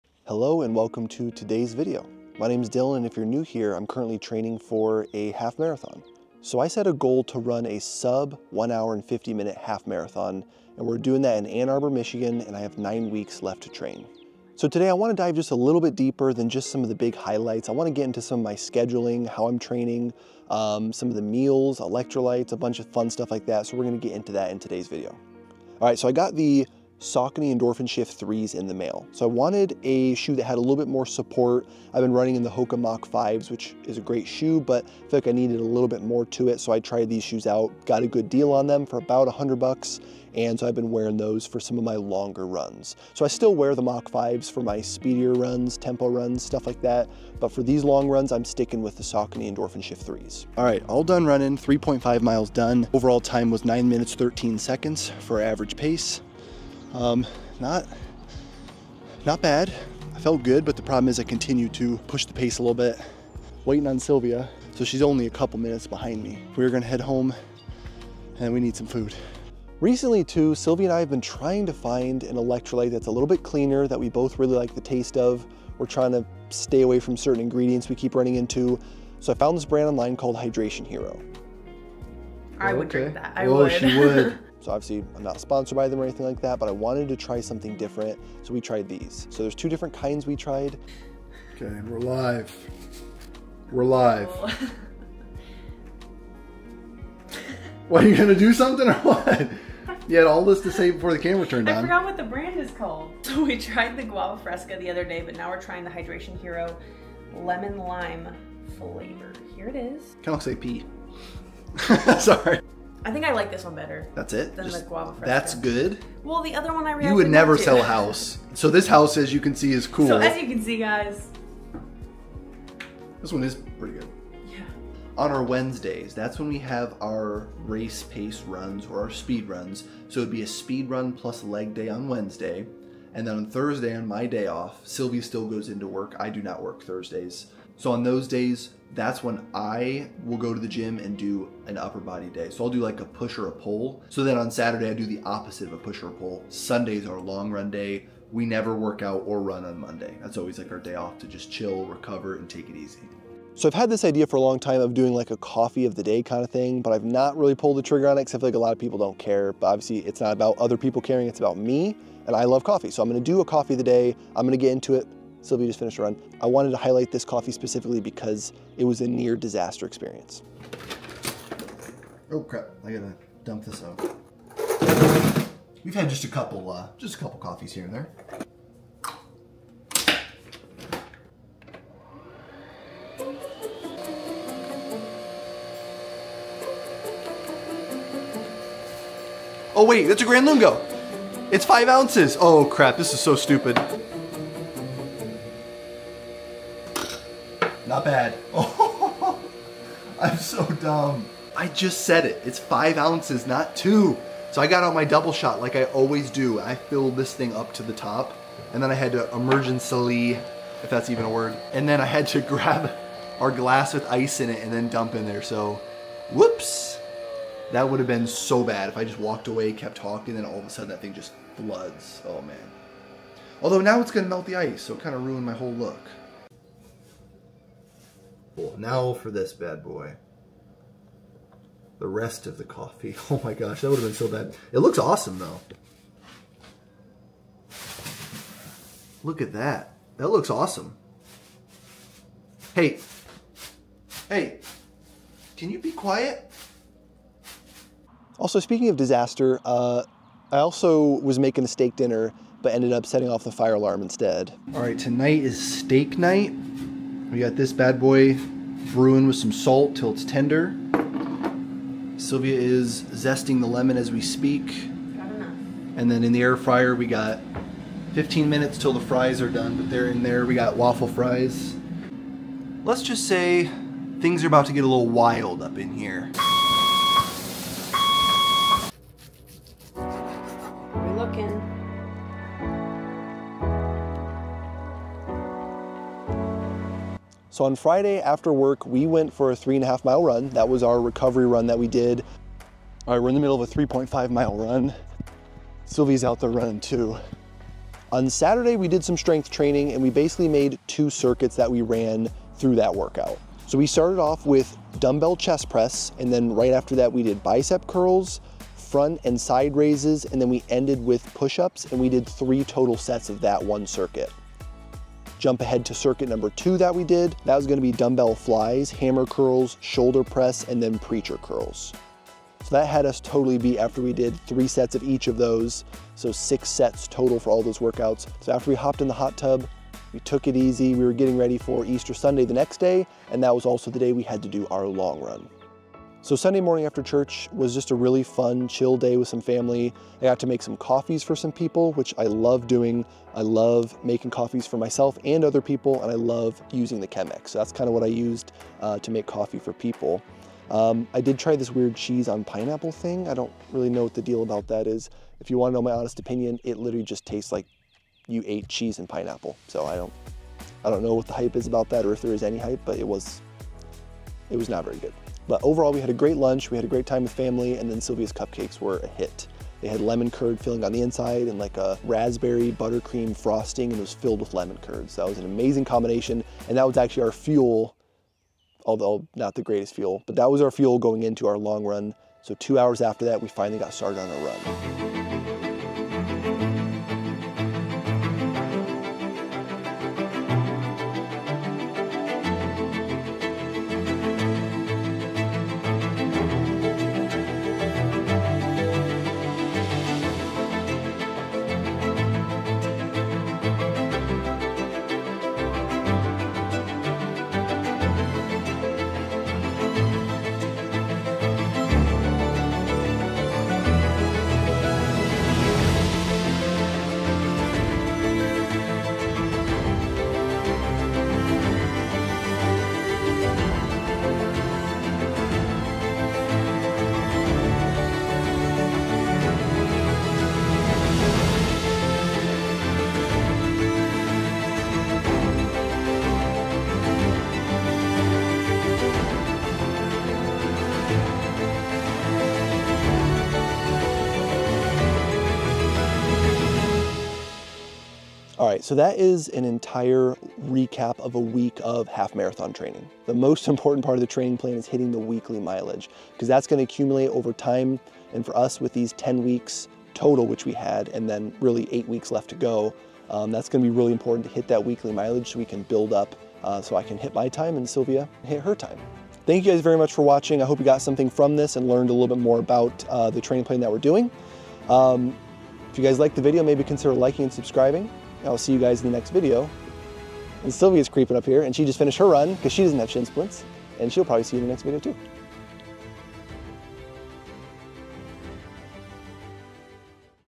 How I'm Training for a Half-Marathon | a full week of training
I'm covering a week in my life as I train for my first Half-Marathon. I almost burnt down the kitchen, tried new electrolytes, got brand-new shoes, and tackled our longest run yet! Join along!

Follow me on Strava: Dillon Berry (Ohio)